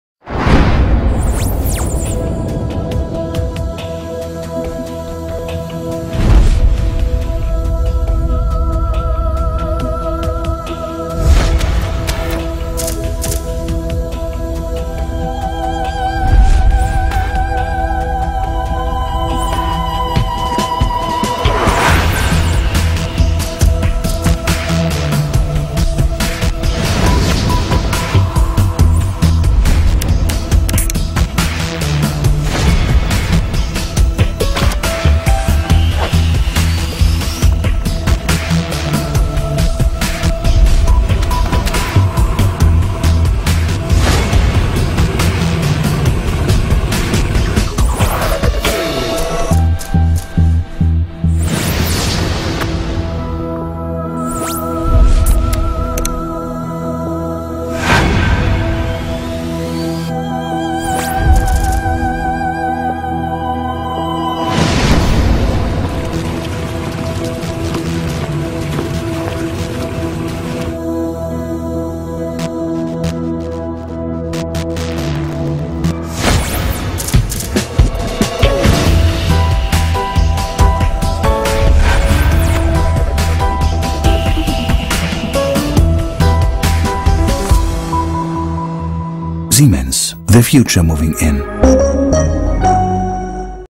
Siemens Varioperfect, bespaar tijd OF energie!.m4v
Tijd besparen of energiezuinig wassen. Altijd met een perfect wasresultaat! Kijk voor alle modellen op